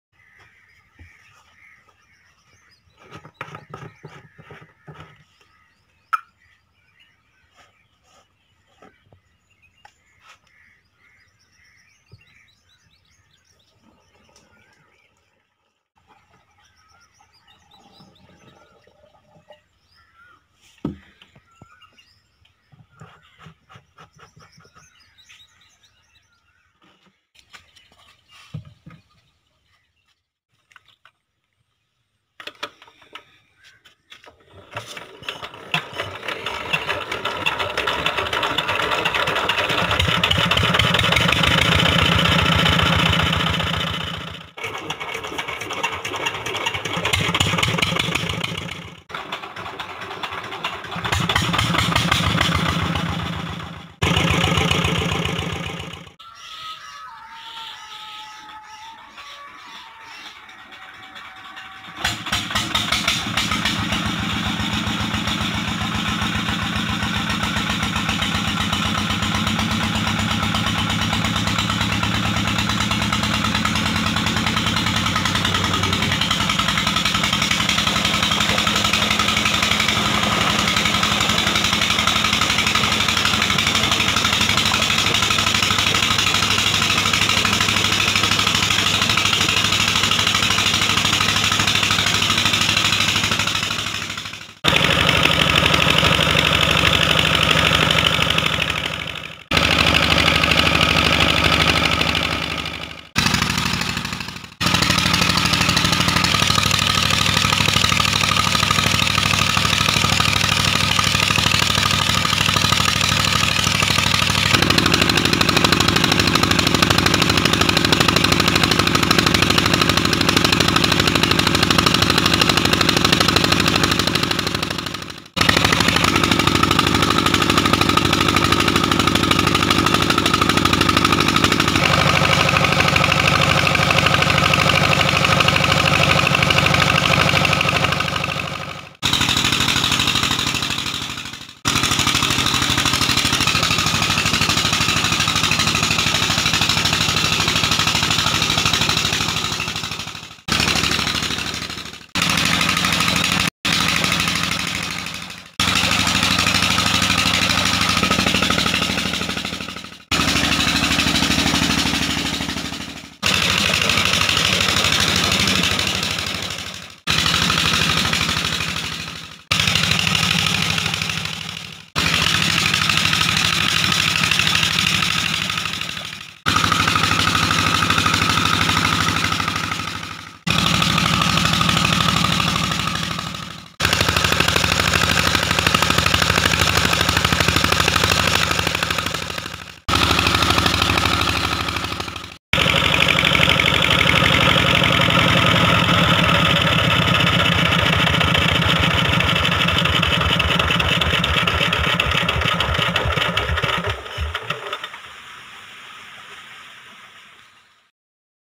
Petter diesel engine 23 hp working on water pump
Petter diesel engine 23 hp working on water pump.video Punjab Fatehpur#Petterdieselengine23hp#petter engine working on water pump#petterengine23hpPunjabFatehpur